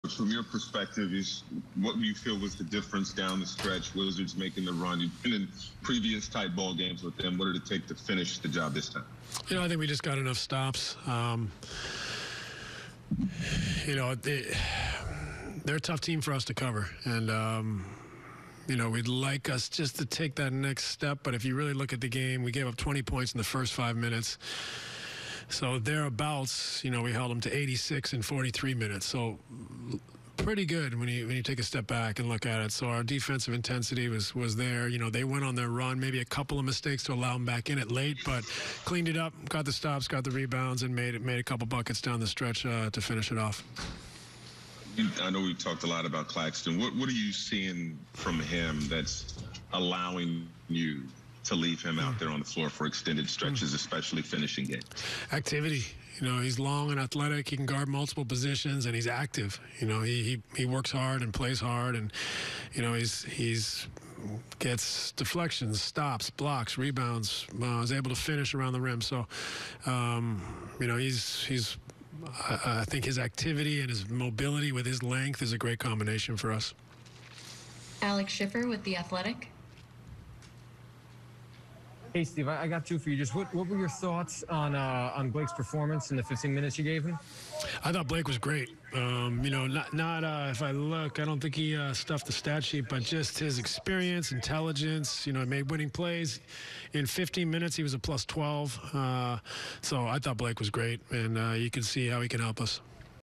Steve Nash breaks down Nets win
Nets coach Steve Nash breaks down the win against Washington.